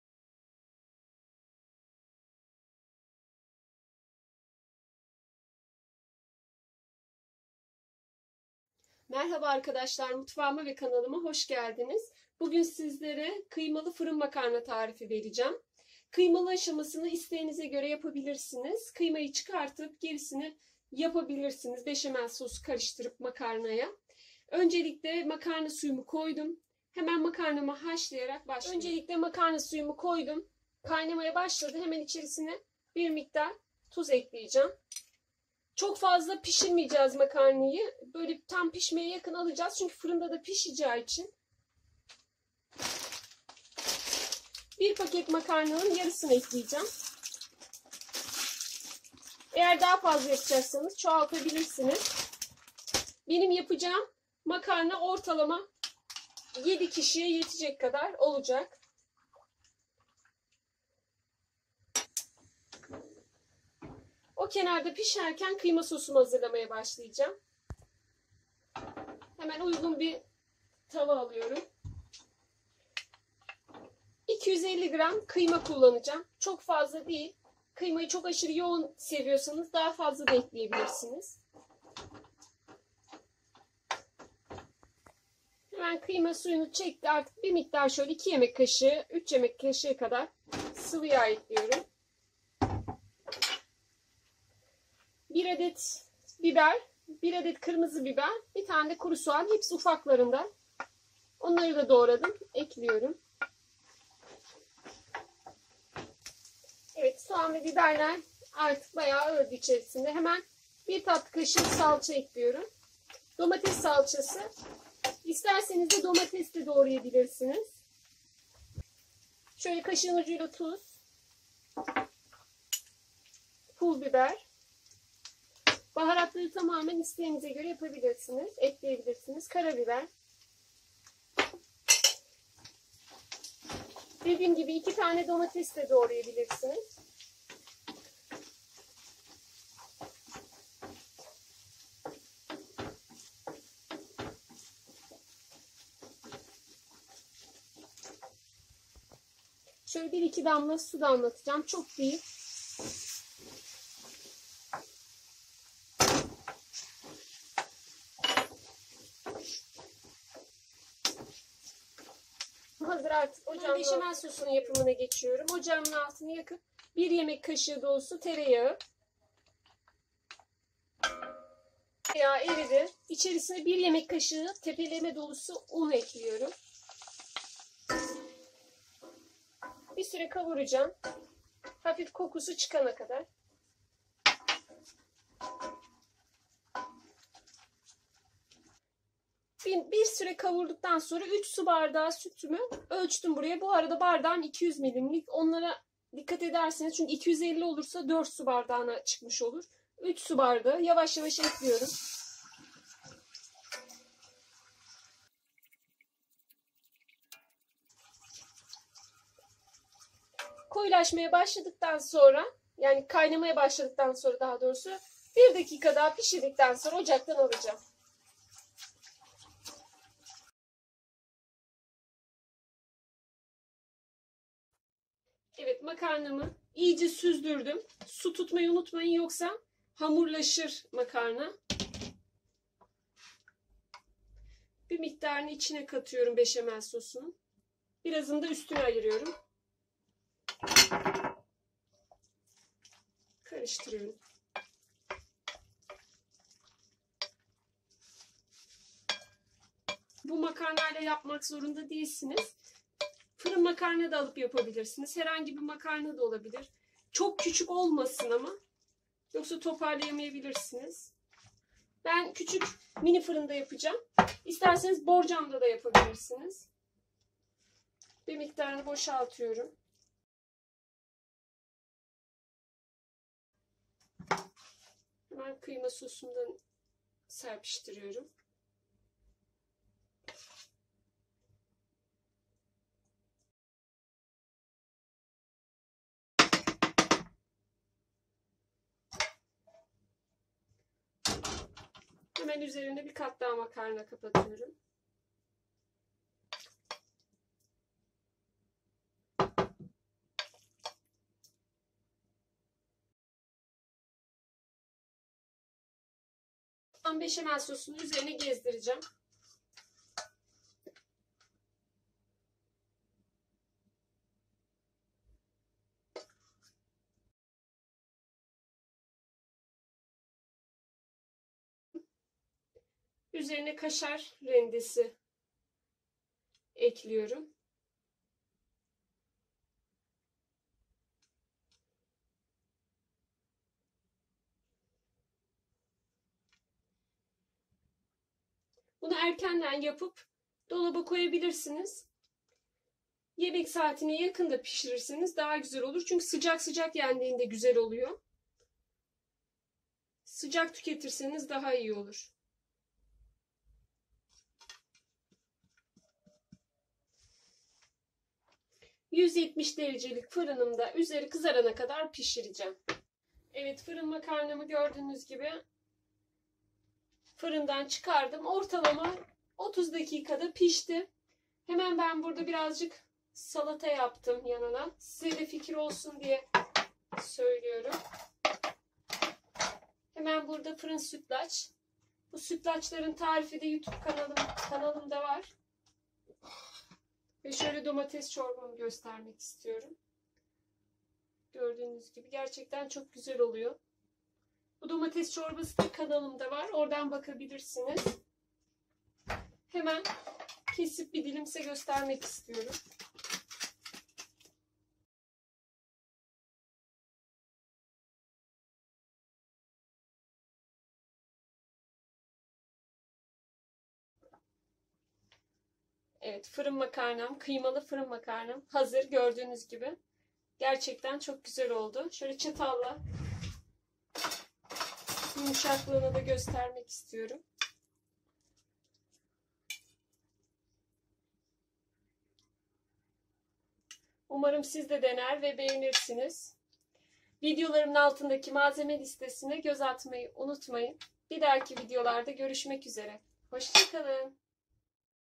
KIYMALI FIRIN MAKARNA(muhakak denemeniz gereken enfes bir tarif)
MALZEMELER

yarım paket burgu makarna kullandım ama siz istediginiz bir makarna çeşidi kullanabilirsiniz.
kıyma harcı için
250 gr kıyma
3 yemek kşığı sıvıyağ
1 adet yeşil biber
1 adet kurusoğan
1 adet kırmızı biber
1 tatlı kaşığı domates salçası veya 2 adet domates
tuz,karabiber,pulbiber
beşemel sosu için
1yemek kaşığı tereyağ
1 yemek kaşığı dolusu un
3 su bardağı süt(200 ml bardak)
tuz,karabiber

üzeri için
yeterince kaşar rendesi
not;video esnasında beşamel sosuna attığım tuz ve karabiberi attığım kısım anlamadığım bir şekilde silinmiş